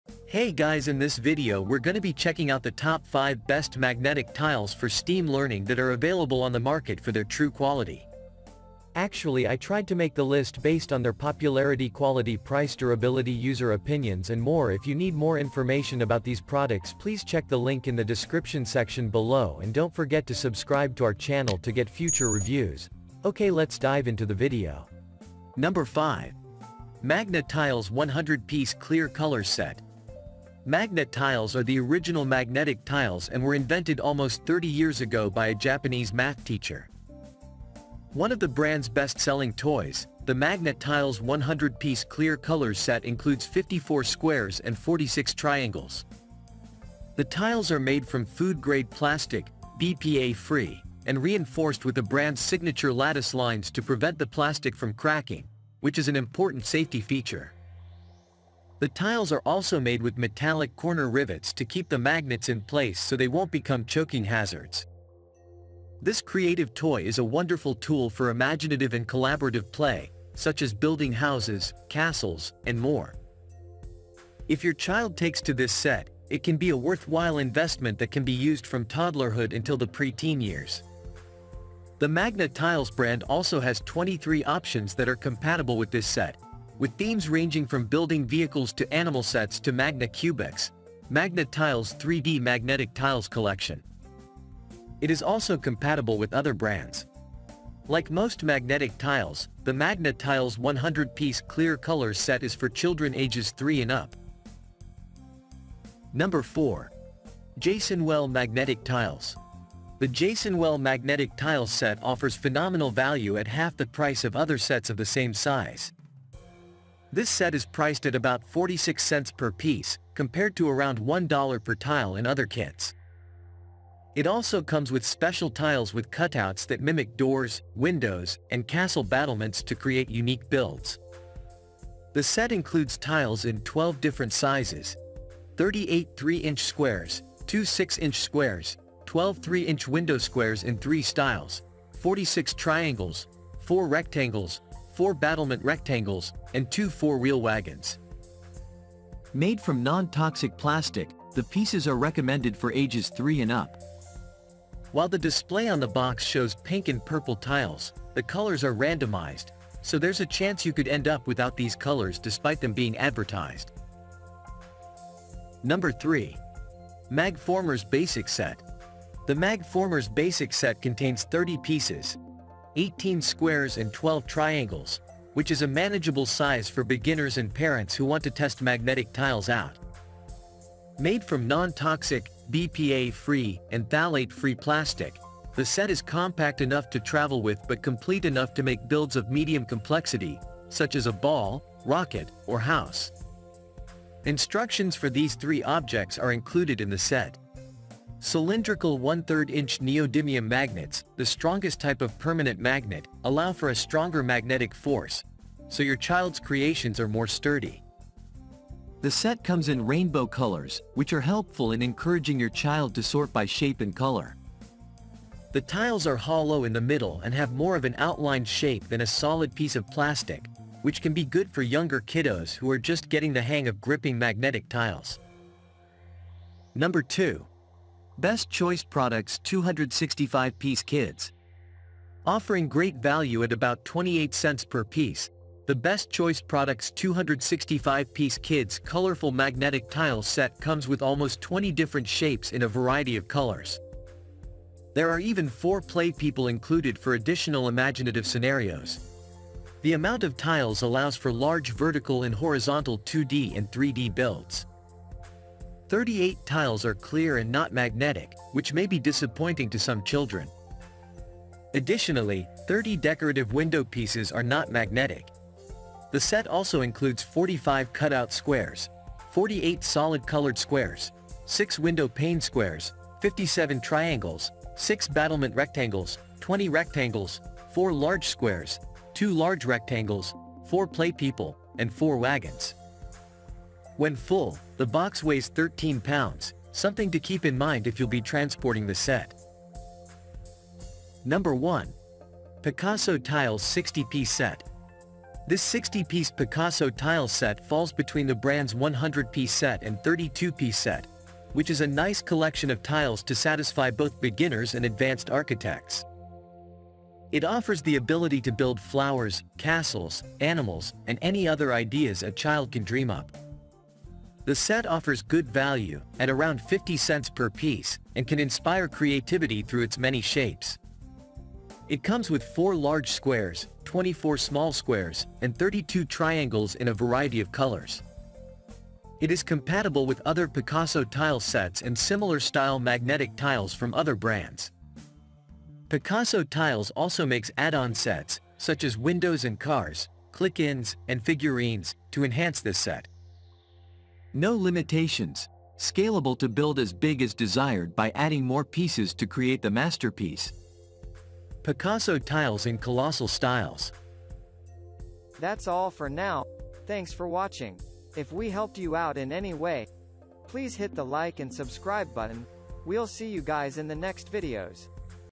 Top 5 Best Magnetic Tiles for STEAM Learning and Creative Play
☛ All The Links to Best Magnetic Tiles for STEAM Learning listed in this Video:-

►5. Magna-Tiles 100-Piece Clear Colors Set.

► 4. Jasonwell Magnetic Tiles.

► 3. Magformers Basic Set.

► 2. Best Choice Products 265-Piece Kids.

►1. PicassoTiles 60 Piece Set.